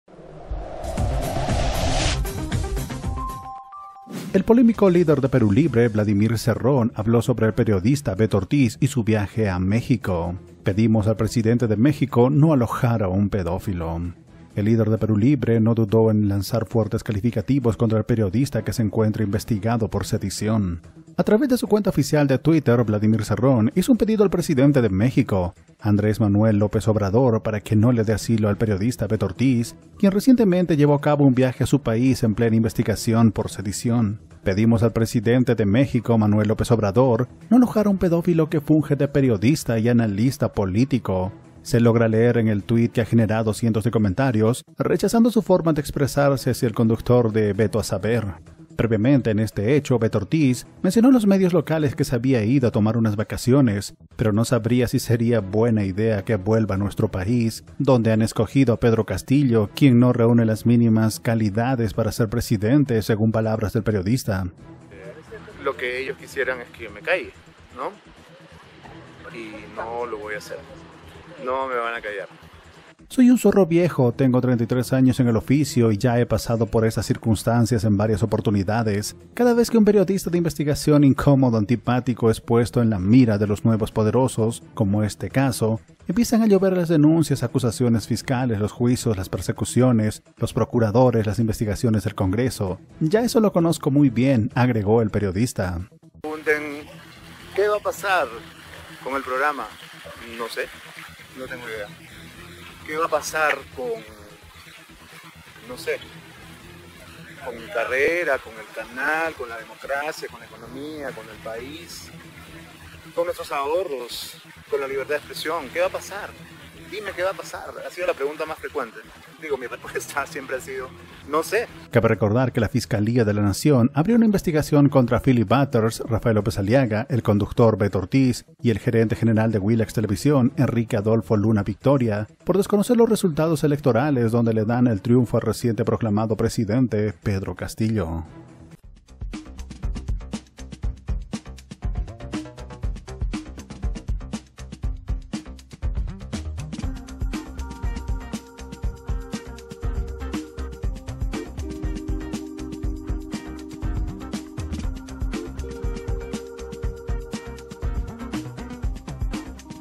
FUERTE!! VLADIMIR CERRON PIDE AL PRESIDENTE DE MEXICO NO DAR ASILO A BETO
A través de su cuenta oficial de Twitter, Vladimir Cerrón hizo un pedido al presidente de México, Andrés Manuel López Obrador para que no le de asilo al periodista Beto Ortiz, quien recientemente llevó a cabo un viaje a su país en plena investigación